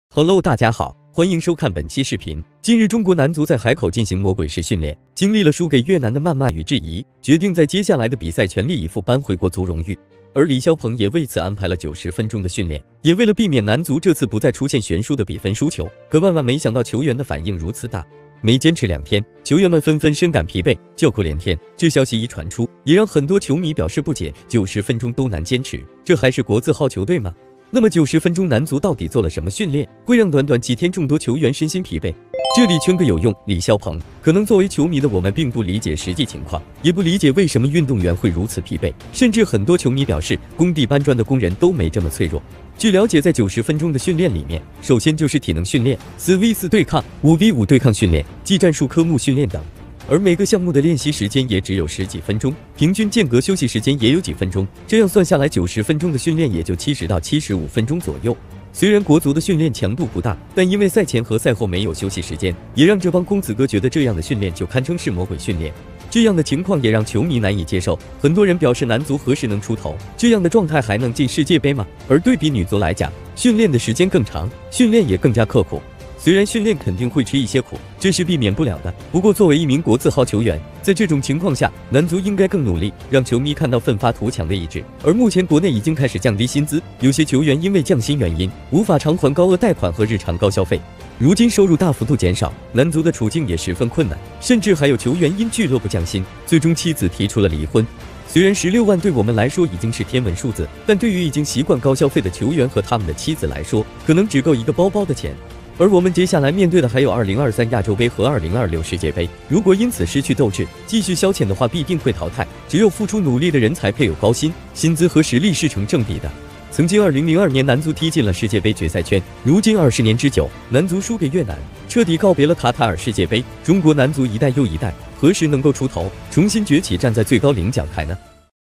男足开展魔鬼式训练，90分钟深感疲惫，这状态能进世界杯吗？
👉每天更新最精彩的体育视频👆点关注不迷路哦 👉👉有问题请私信
体育领域创作者